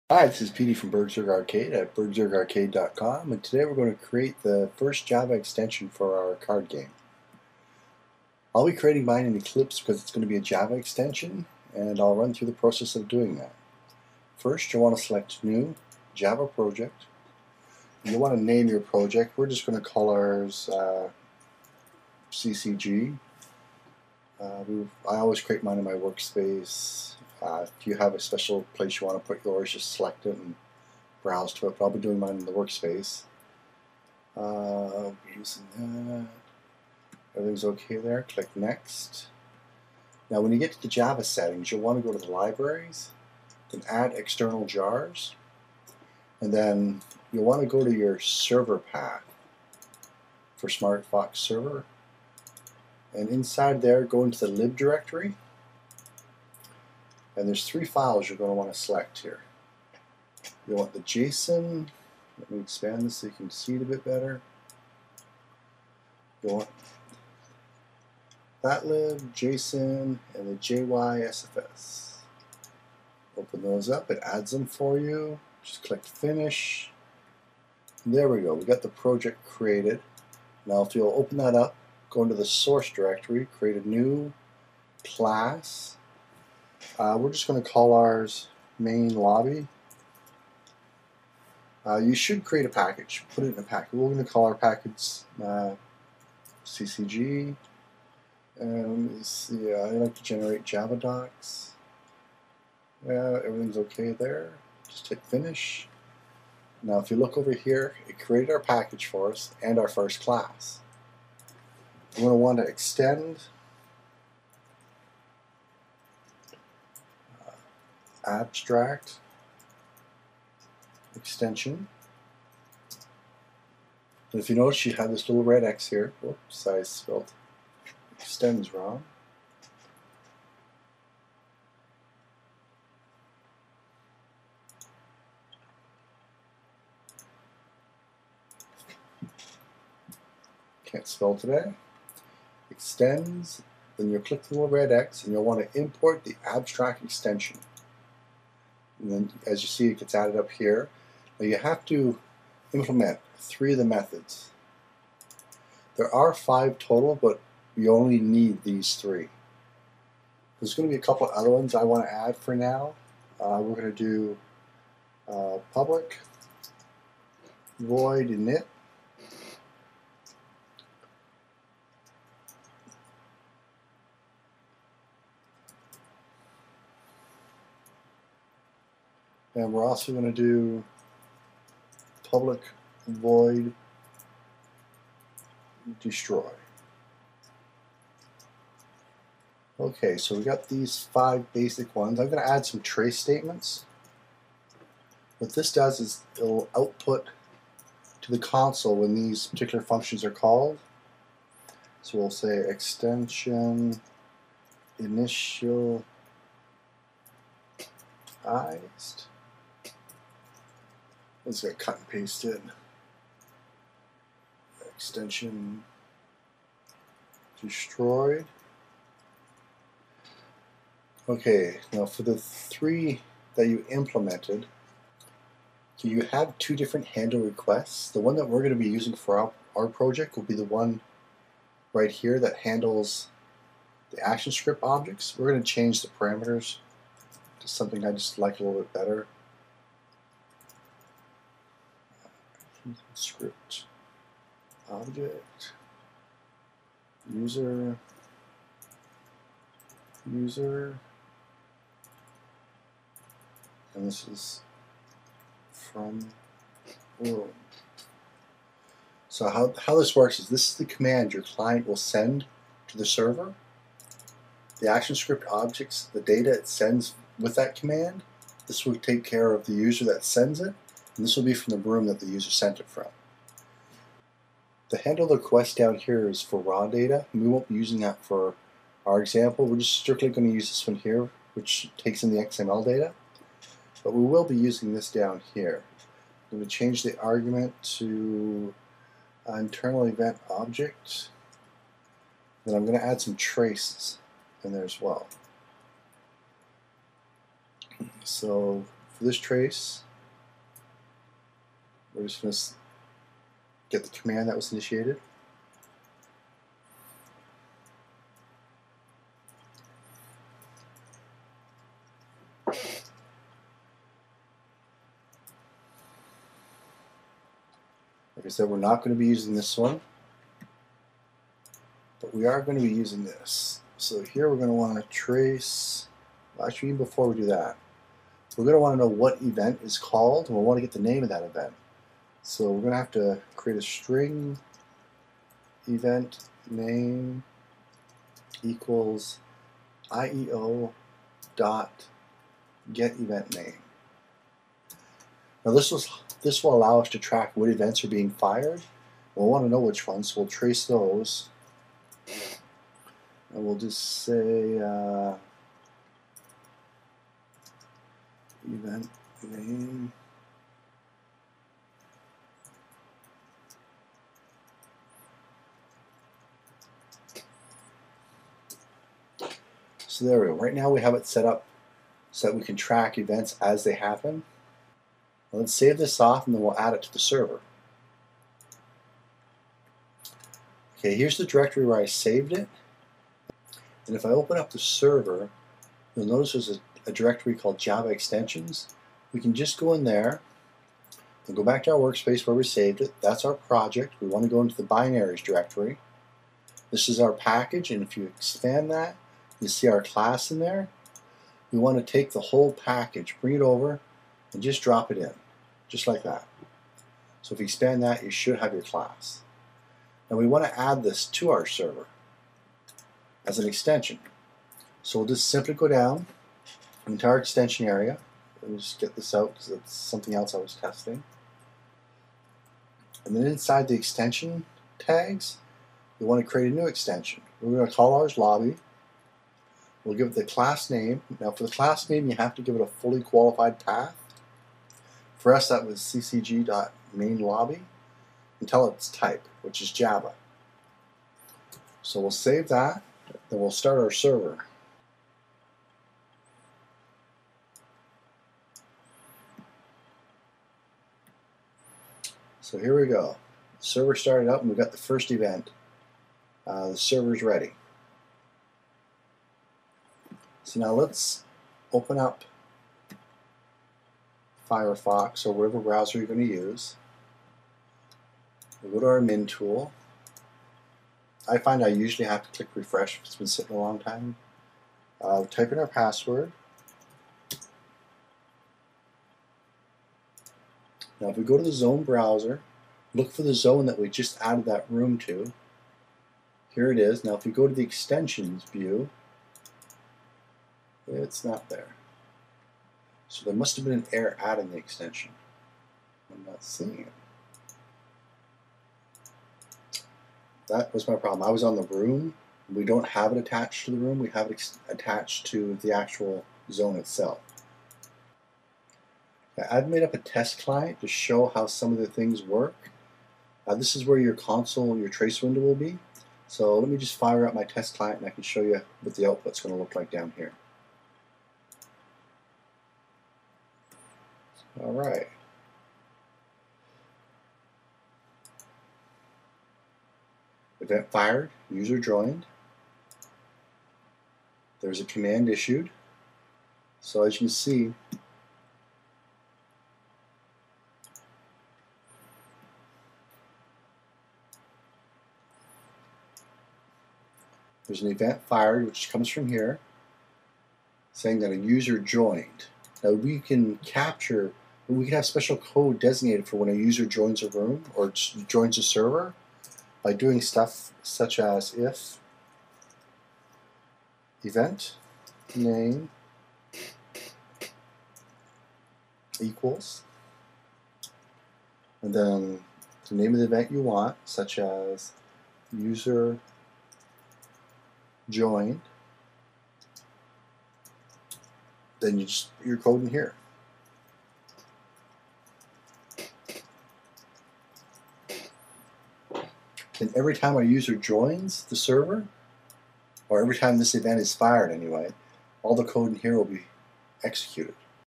SmartFoxSrver - Creating Our Java Extentsion, Part 1
In this tutorial we will start the process of creating our java extention for smartfoxserver pro.